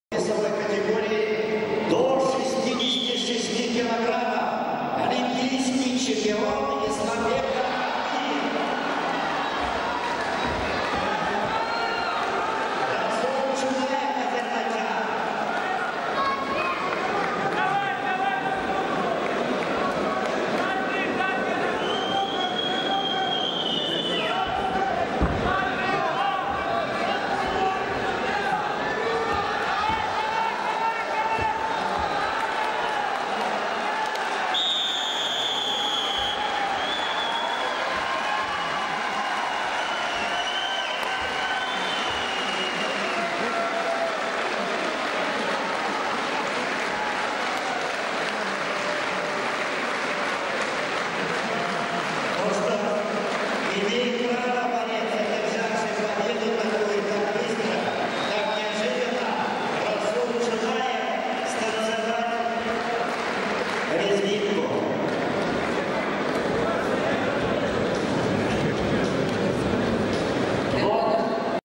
Best Dance in Wrestling (Gold Medal Match) GRAPPLERSPLANET.COM
Kazan-2013 / Rasul Chunayev (AZE) - Islambek Albiev (RUS) - gold medal match
Amazing.
AZERBAIJAN FOLK DANCE
Well. Now everyone in the Olympics needs to up their victory dancing game. This was really cool to watch.